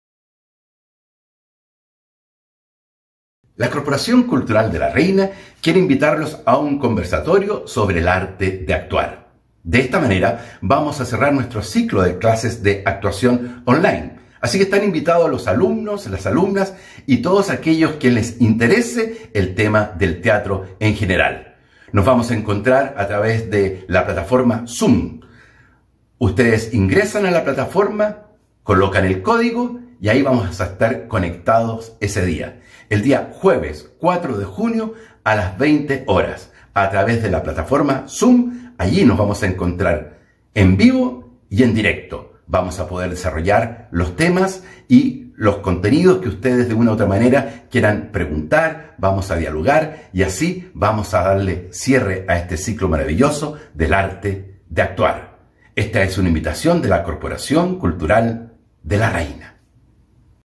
Invitación Conversatorio con Rolando Valenzuela: El Arte de Actuar y Cierre Taller Online
Conversatorio con Rolando Valenzuela: El Arte de Actuar y Cierre Taller Online
Jueves 4 de Junio, 20:00 hrs

ID de reunión: 👉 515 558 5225
Contraseña: 👉 6W3PZp

La Corporación Cultural de la Reina desea dar un gran cierre a este curso de actuación a cargo del gran director y actor Rolando Valenzuela.

¡Todos invitados!